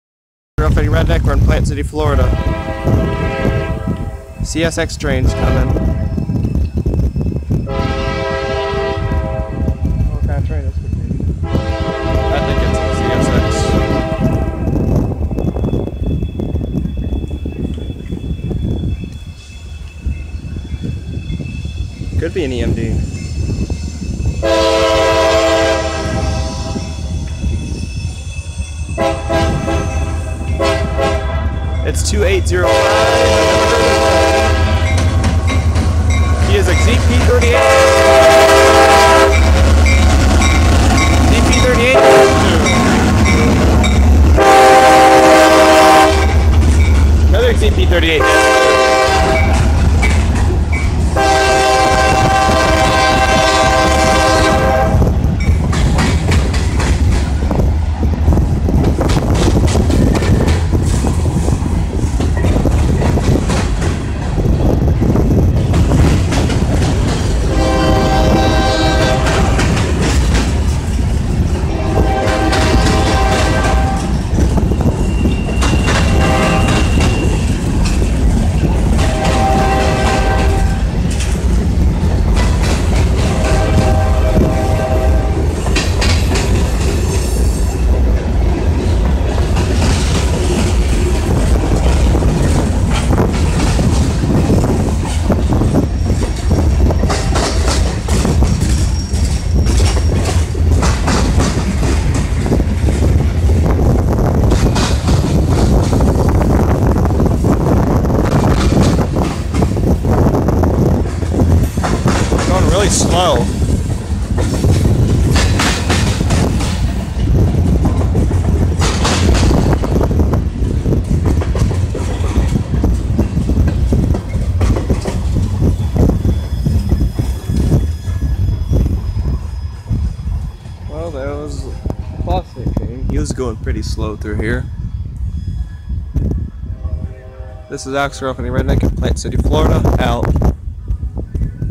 Yes, I'm back on YouTube.
This is a video I made in Plant city Florida of a CSX train going pretty slow past the station. And this is the first video I uploaded to here in over a month. So my vaction from YouTube is over.